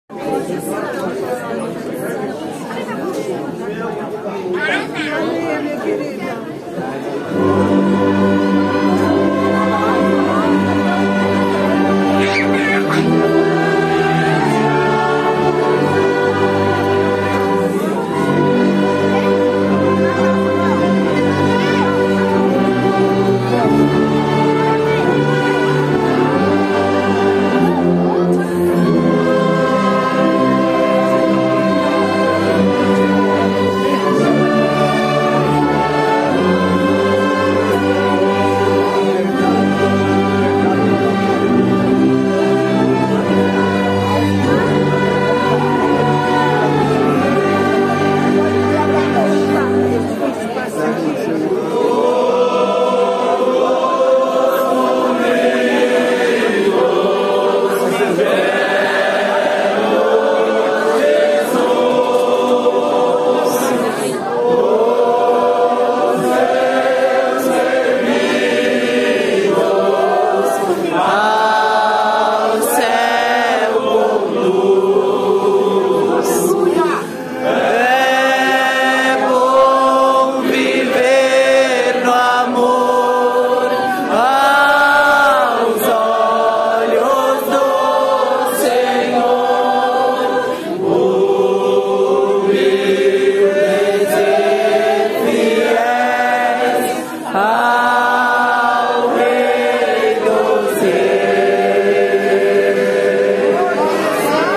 ENSAIO LOCAL CCB BAIXADA DO GLICÉRIO CENTRO-SP
DIA INESQUECÍVEL 😭😭 ESTAVA LOTADO ENSAIO LOCAL CCB BAIXADA DO GLICÉRIO CENTRO-SP